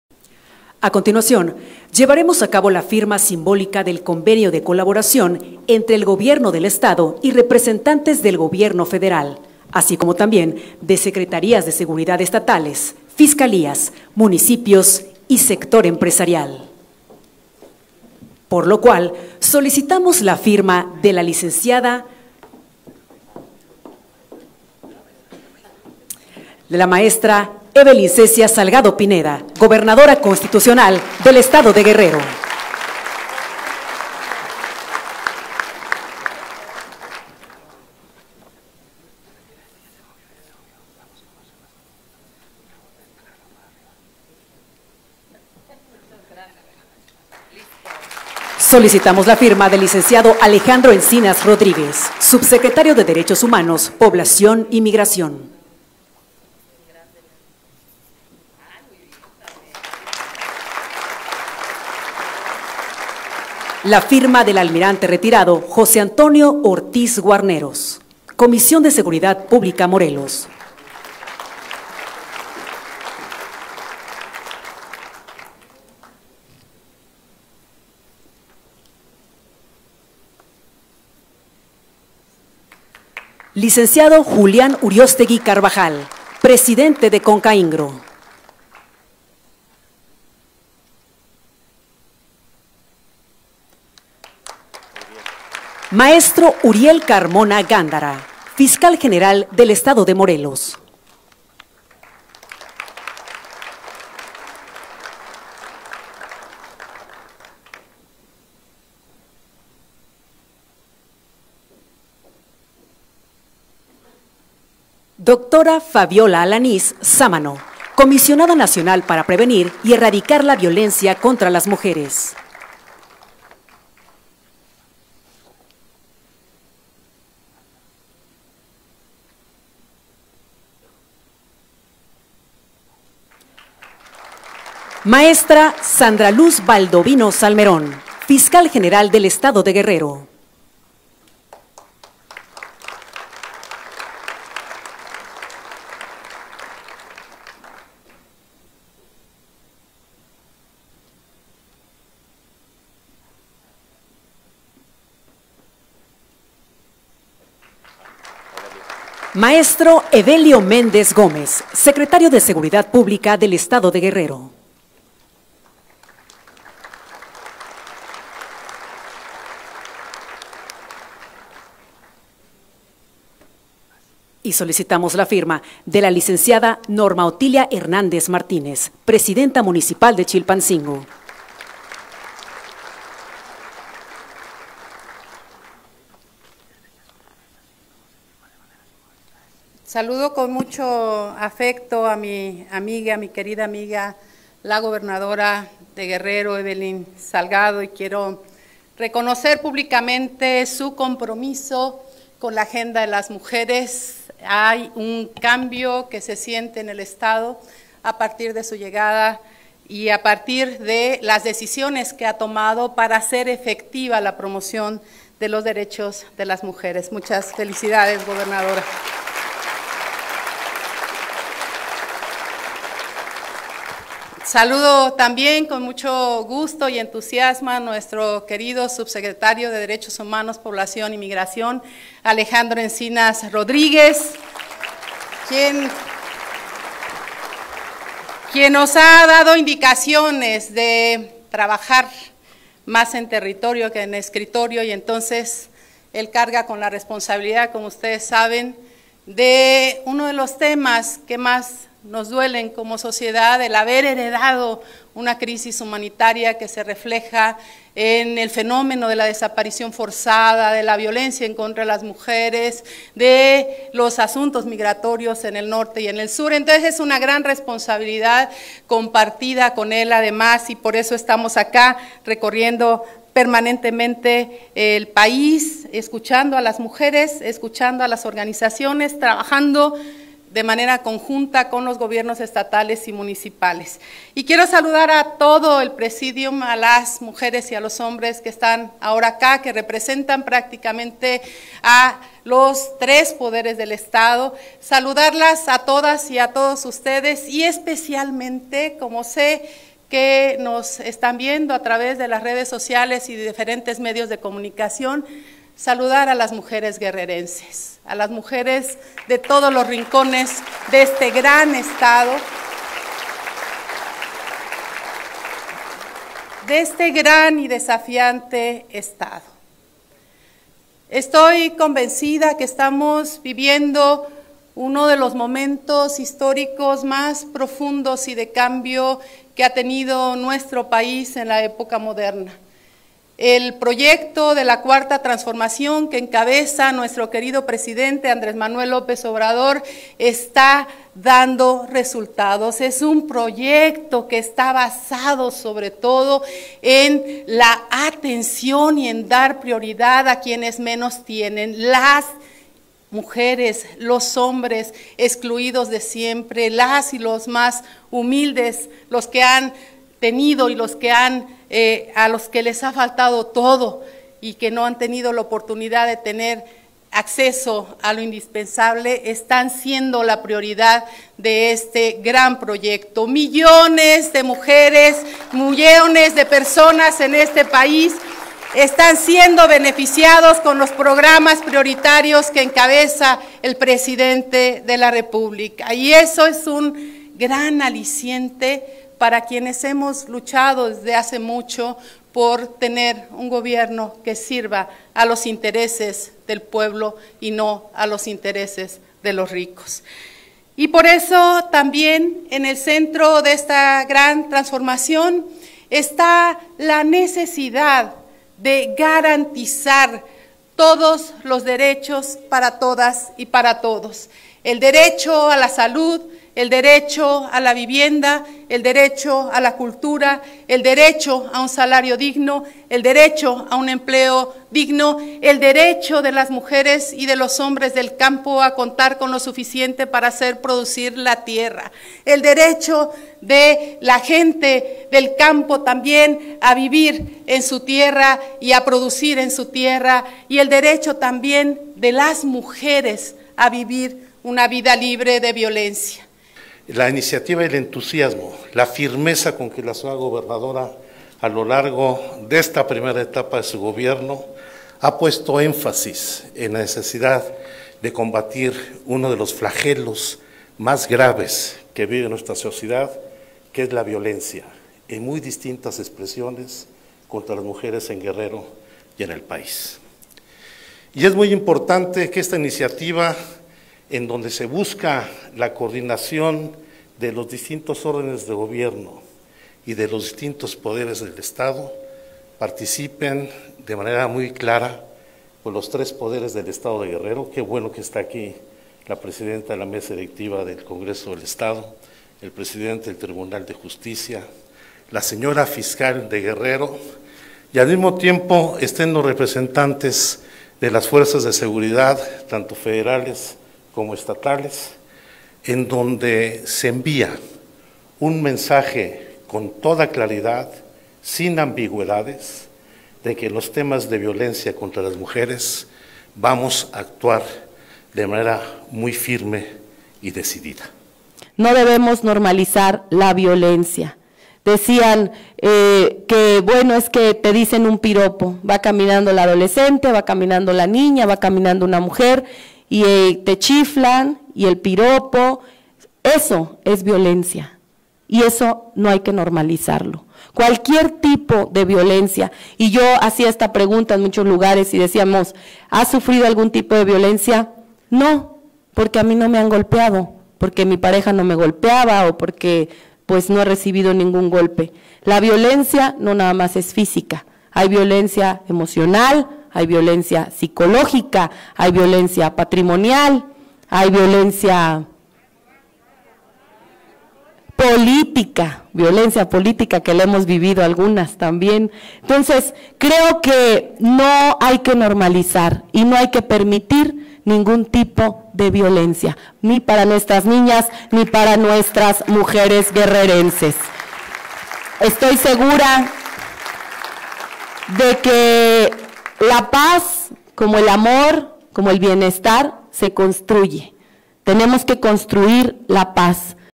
Firma de Protocolo Violeta
A fin de implementar la Alerta Violeta que pretende la inmediata búsqueda y localización de niñas, mujeres adolescentes y adultas reportadas como desaparecidas y en posible situación de riesgo, la SEGOB, la Fiscalía de Morelos y el gobierno de Guerrero firmaron un convenio que une esfuerzos de difusión informativa.

La firma de este convenio de colaboración, entre Alejandro Encinas, Carmona Gándara y la Gobernadora de Guerrero, Evelyn Salgado, generará los instrumentos jurídicos para la implementación del protocolo Alerta Violeta, así como para impulsar acciones conjuntas que faciliten la búsqueda y localización de personas.

También firmaron el "Protocolo Violeta: cada minuto cuenta", Fabiola Alanís Sámano, Comisionada Nacional para Prevenir y Erradicar la Violencia contra las Mujeres; Sandra Luz Valdovinos Salmerón, Fiscal General del Estado de Guerrero; Evelio Méndez Gómez, secretario de Seguridad Pública de Guerrero; Norma Otilia Hernández Martínez, Alcaldesa de Chilpancingo; Julián Urióstegui Carbajal, titular de Concanaco Guerrero, y José Antonio Ortiz Guarneros, Comisionado Estatal de Seguridad Pública de Morelos.